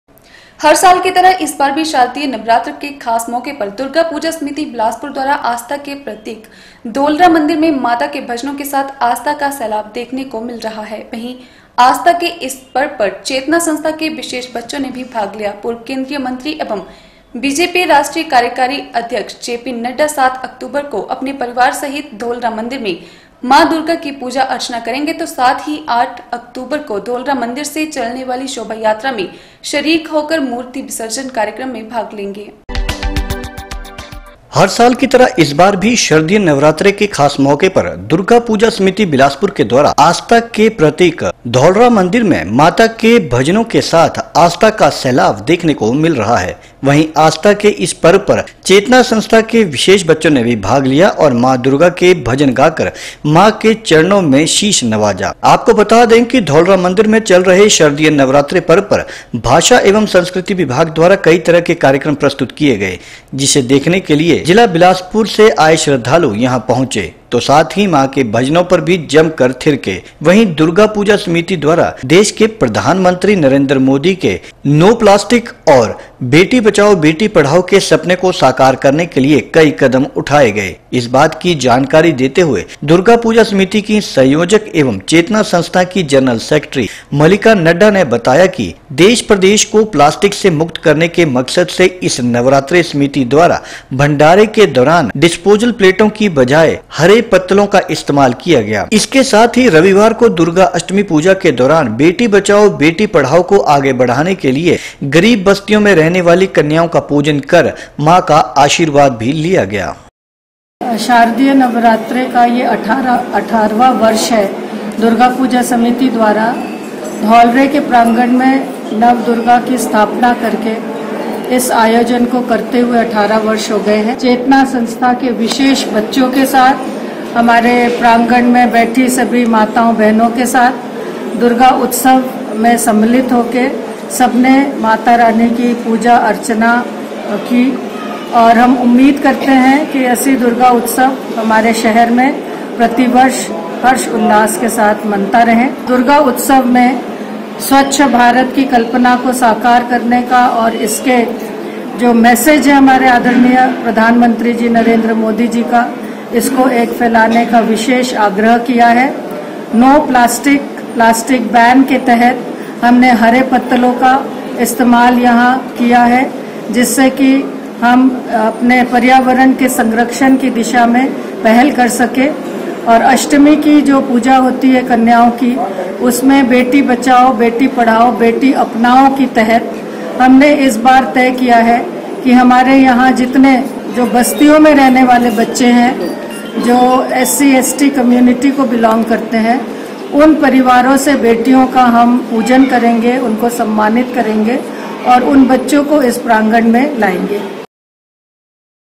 7 OCT N 5 In the Dholra temple, there is a flood of faith with the hymns of the mother.
हर साल की तरह इस बार भी शारदीय नवरात्र के खास मौके पर दुर्गा पूजा समिति बिलासपुर द्वारा आस्था के प्रतिक धौलरा मंदिर में माता के भजनो के साथ आस्था का सैलाब देखने को मिल रहा है. वहीँ आस्था के इस पर्व पर चेतना संस्था के विशेष बच्चों ने भी भाग लिया पूर्व केंद्रीय मंत्री एवं बीजेपी राष्ट्रिय कार्यकारी अध्यक्ष जेपी नड्डा 07 अक्टूबर को अपने परिवार सहित धौलरा मंदिर में माँ दुर्गा की पूजा अर्चना करेंगे तो साथ ही 08 अक्टूबर को धौलरा मंदिर से चलने वाली शोभा यात्रा में शरीक होकर मूर्ति विसर्जन कार्यक्रम में भाग लेंगे।